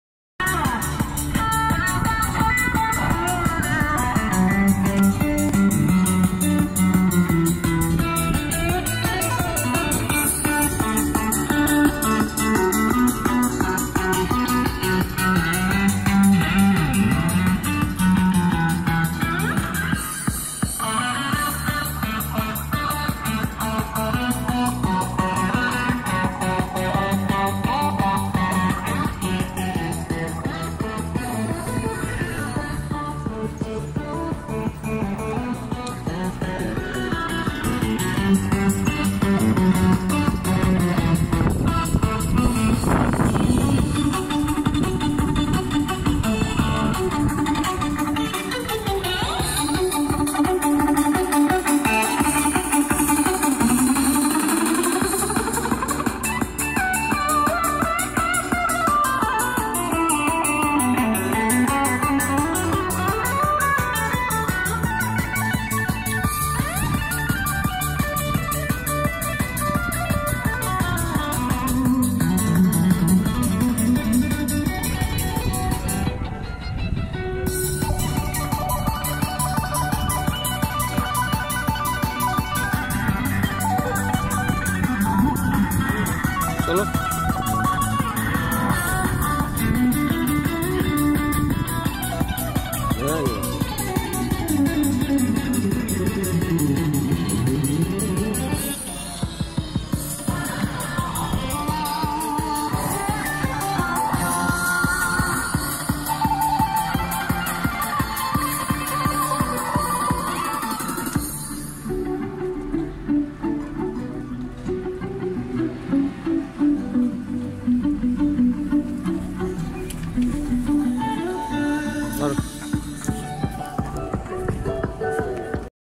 Glorious Spring Season, King Street, South Shields.
Happy Easter Holiday 2021. People's are enjoying lovely Weather.